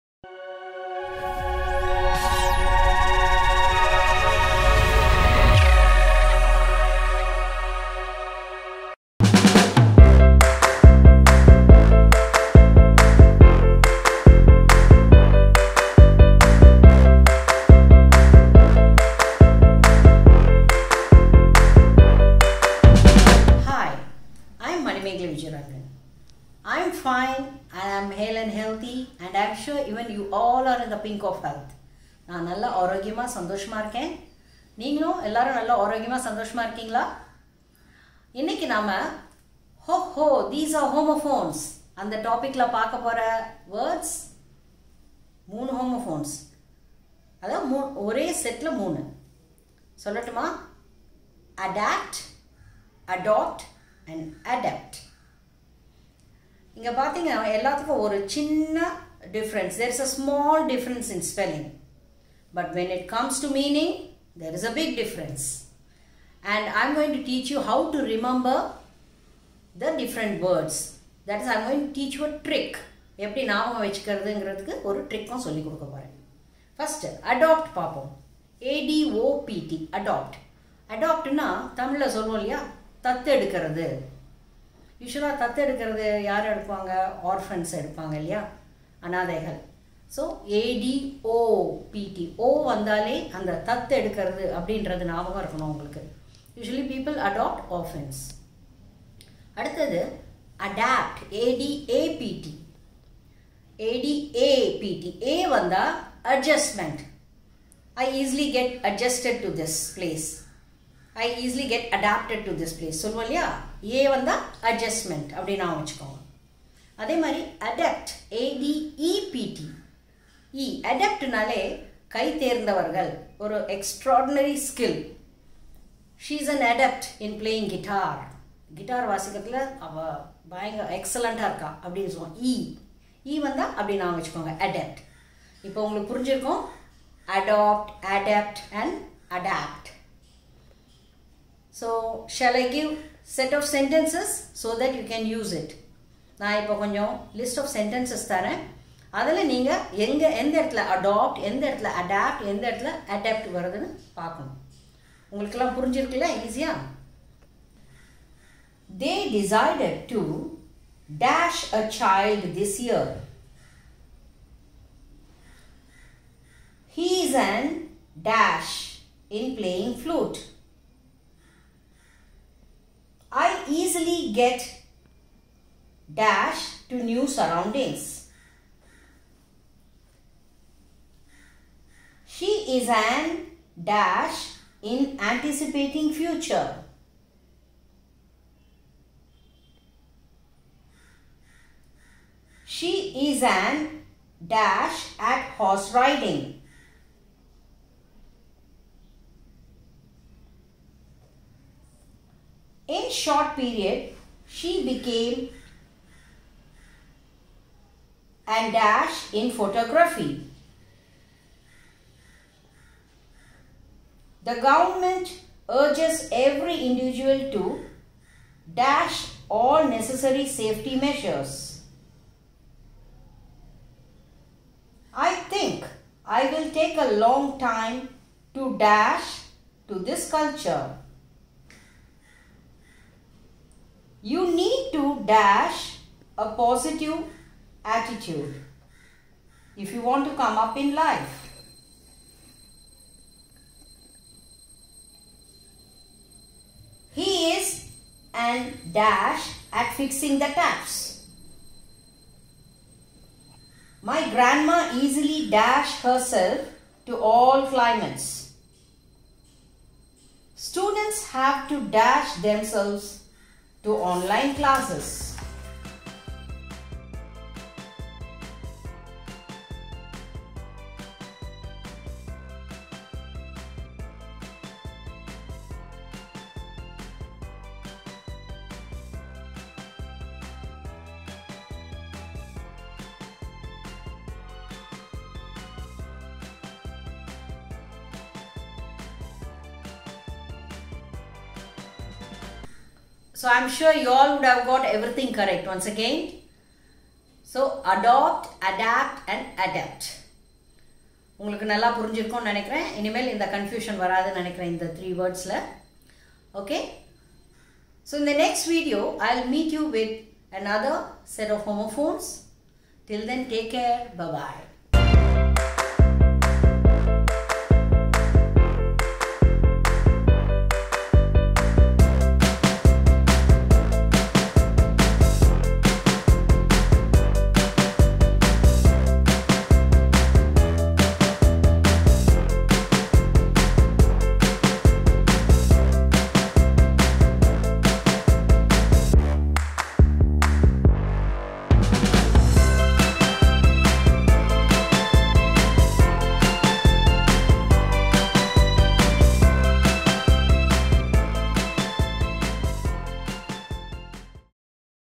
HOMOPHONES- ADAPT, ADOPT, ADEPT | GRAMMAR IN TAMIL | தமிழ் வழியில் ஆங்கிலம் கற்கலாம்
This video explains about the HOMOPHONES- ADAPT, ADPOT, ADEPT through the medium of TAMIL, watch this simple and easy lesson to learn important grammar.

I hope you enjoyed this lesson. Let me know if you have any questions.